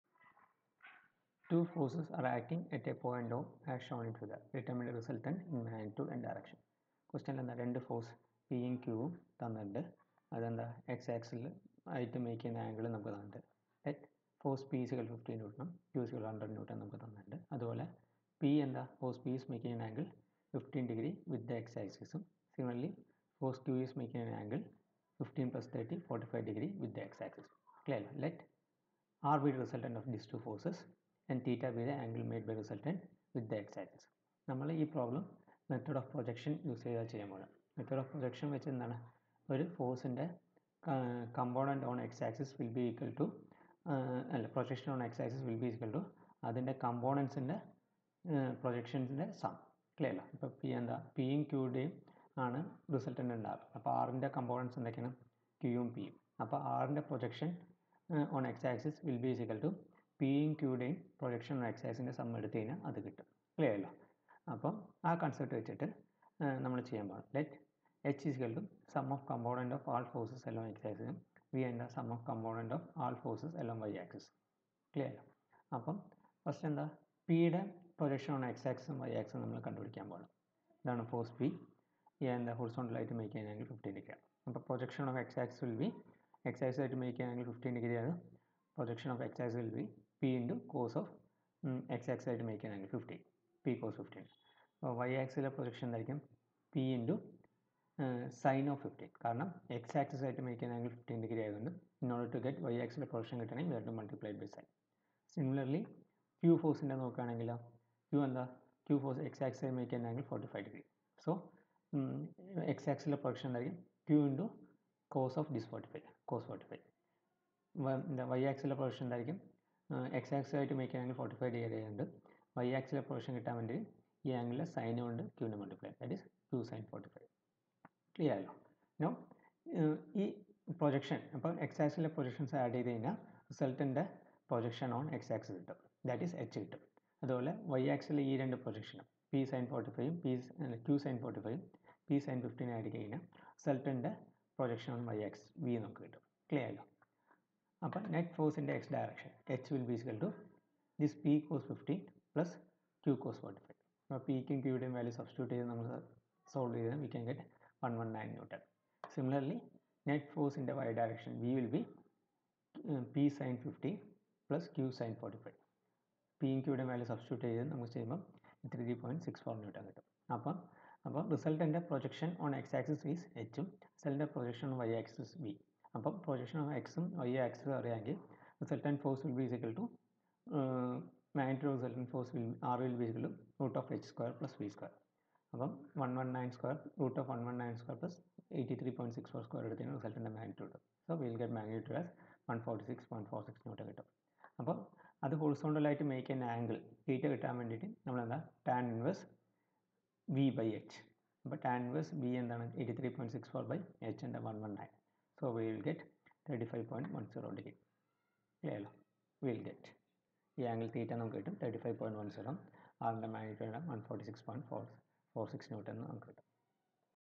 1.6 Resolution of force
APJAKTU EST 100 Engineering Mechanics Practice Problem.

Q) Two forces are acting at a point O as shown in figure. Determine the resultant in magnitude and direction.